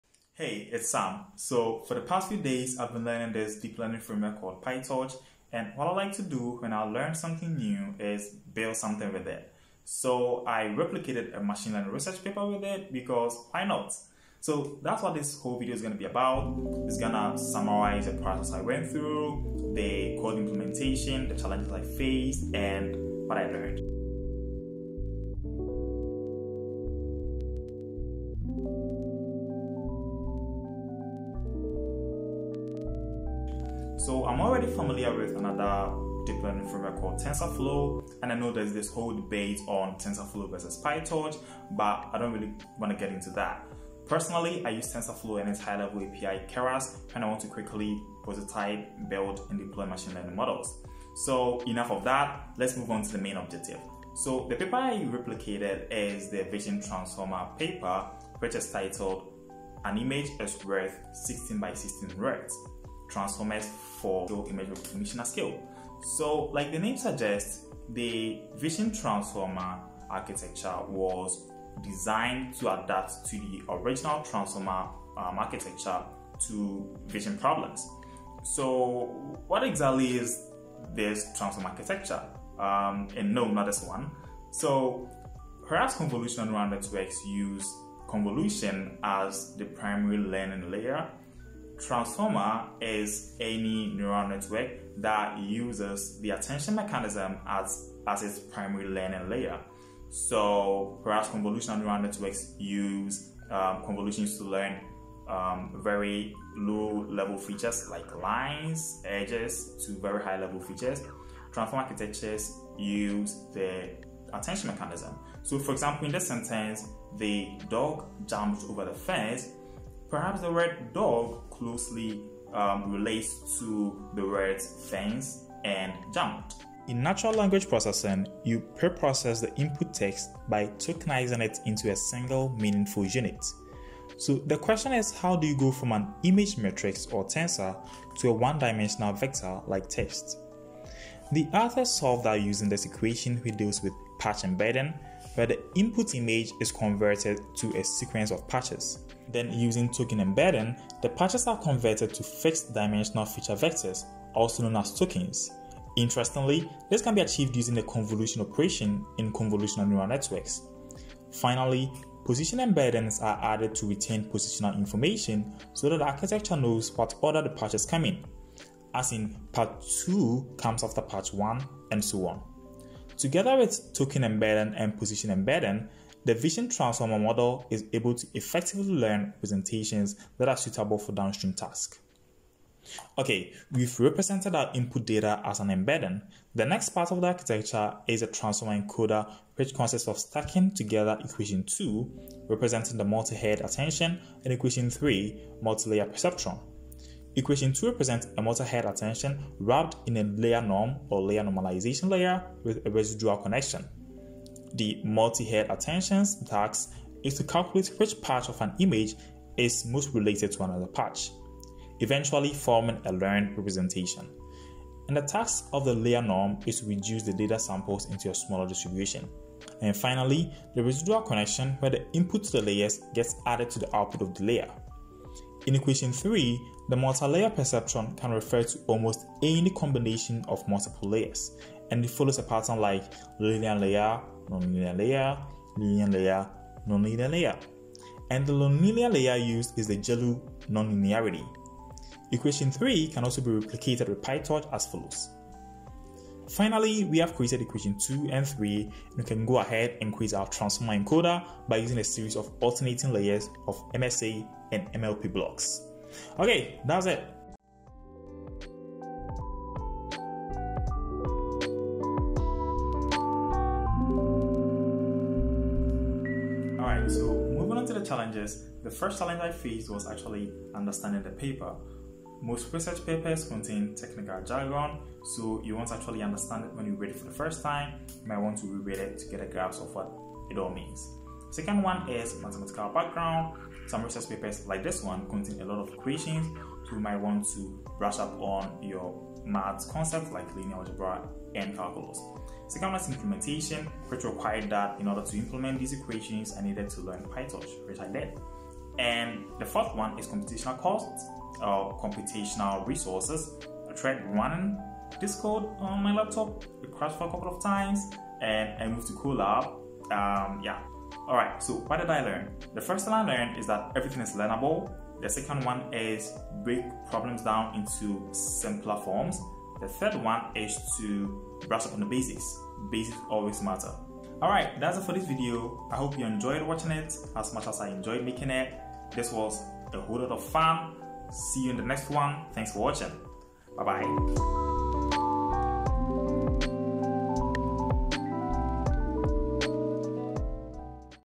Replicating a Research Paper as a CS Undergrad
I replicated the code for a research paper. Here's what I learned.